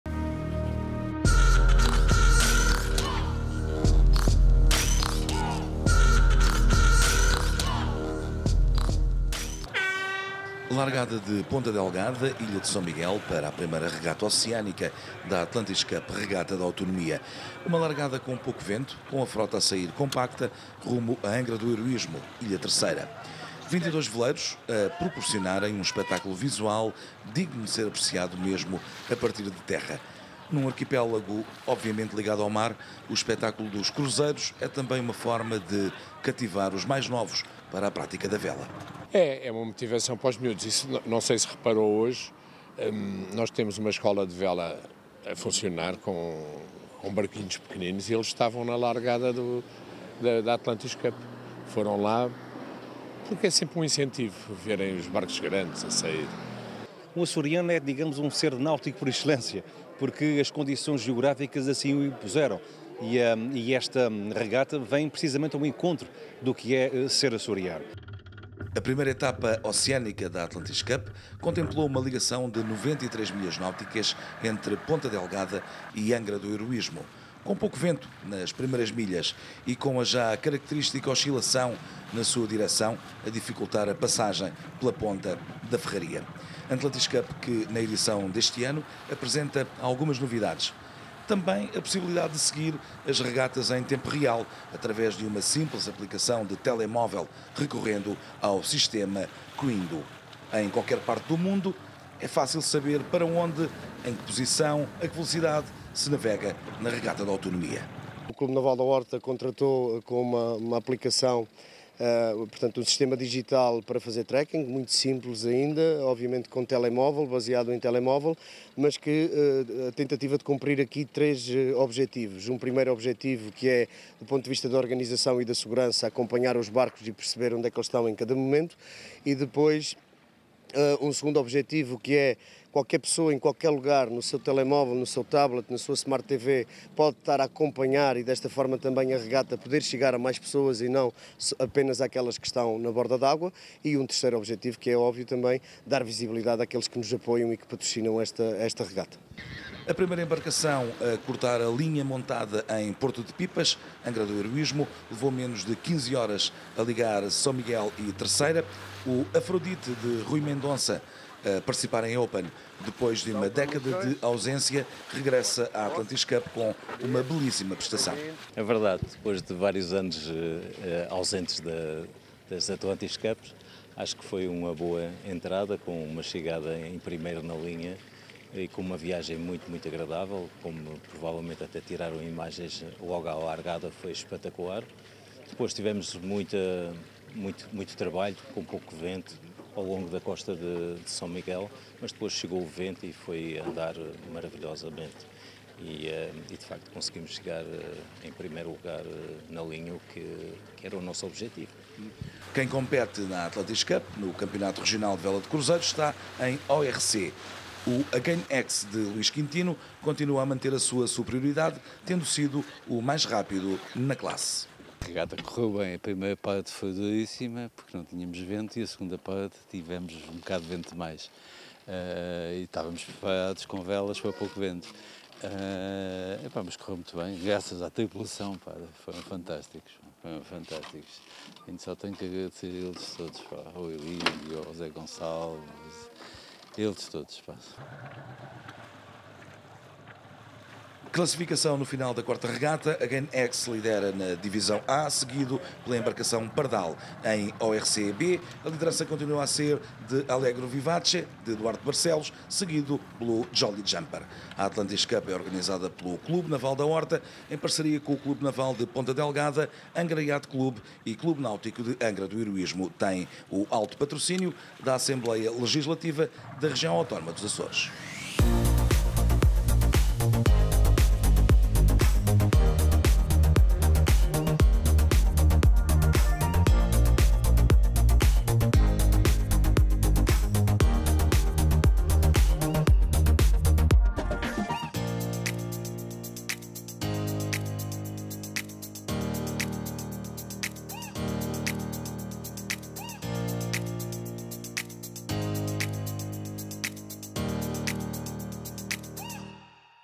Atlantis Cup® - Regata da Autonomia 2022 - Resumo 2º dia
“Again X” de Luís Quintino mantém liderança em ORC A. “Afrodite” de Rui Mendonça foi o mais rápido.
A frota da Atlantis Cup®, Regata da Autonomia, completou a primeira etapa oceânica da edição de 2022, ligando Ponta Delgada (São Miguel) e Angra do Heroísmo (Terceira).

Prémio de Excelência Desportiva 2011
Insígnia Autonómica de Mérito Cívico 2017
Medalha de Mérito Municipal Dourada 2022